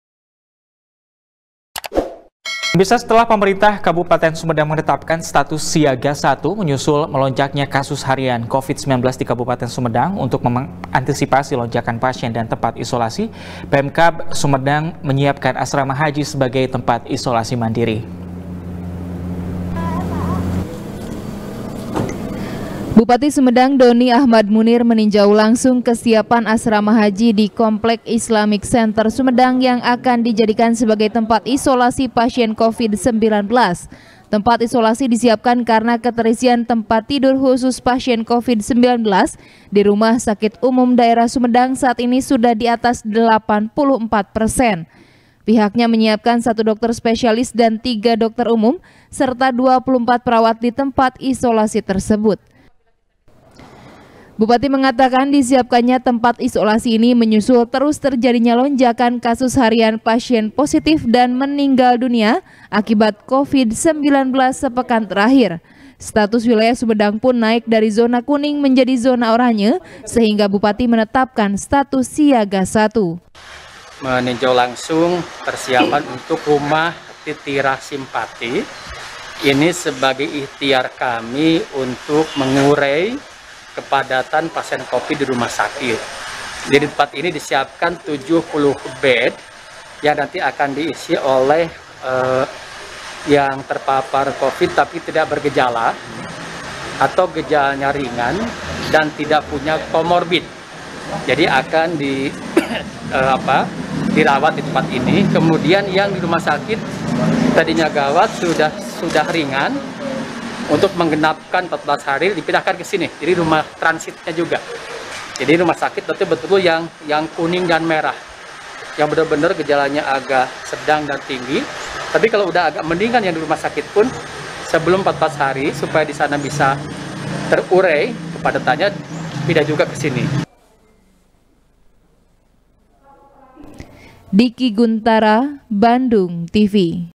Antisipasi Lonjakan Pasien, Pemkab Sumedang Siapkan Tempat Isolasi Di Asrama Haji | SBR | BANDUNGTV
SBR | BANDUNGTV | IRFAN RAMANDA | Setelah Pemerintah Kabupaten Sumedang Menetapkan Status Siaga Satu, Menyusul Melonjaknya Kasus Harian Covid 19 Di Kabupaten Sumedang, Untuk Mengantisipasi Lonjakan Pasien Dan Tempat Isolasi, Pemkab Sumedang Menyiapkan Asrama Haji Sebagai Tempat Isolasi Mandiri.